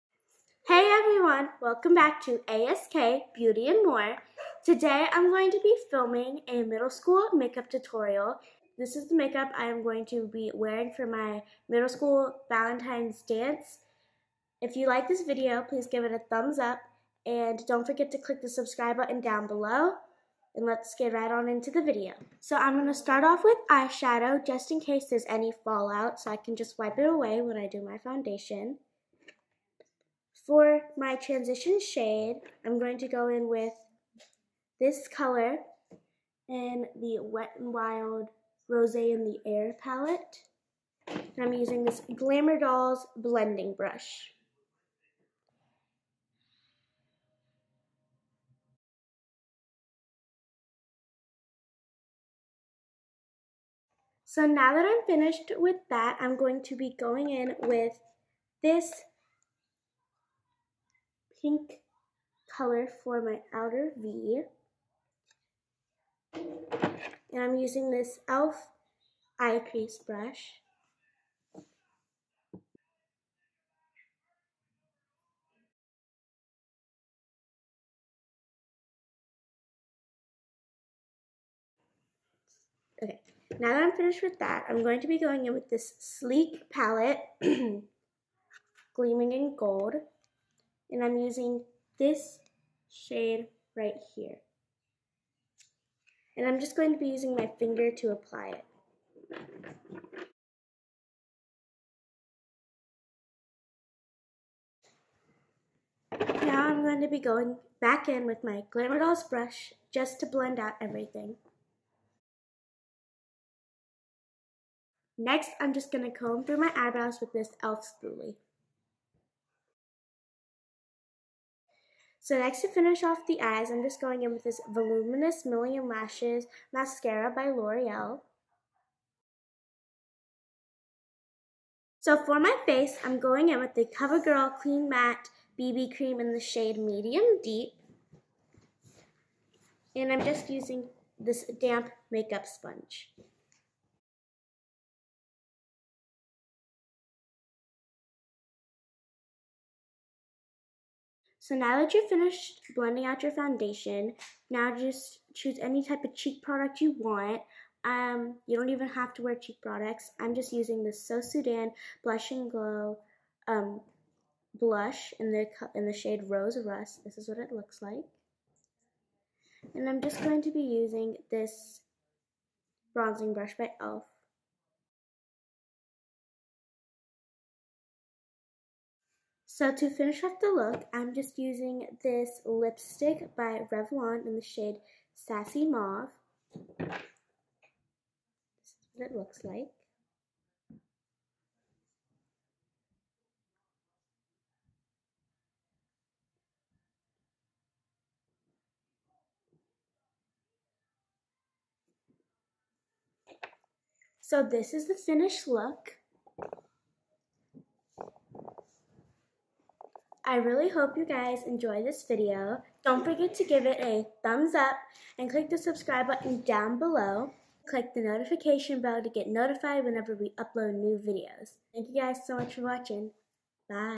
Middle School Dance Makeup Tutorial | Valentine's Makeup
Hey everyone! Today's video is a makeup tutorial Kerri has filmed for her middle school Valentine's dance. We hope you enjoy this video. We would greatly appreciate it.
Xoxo ASK Beauty and More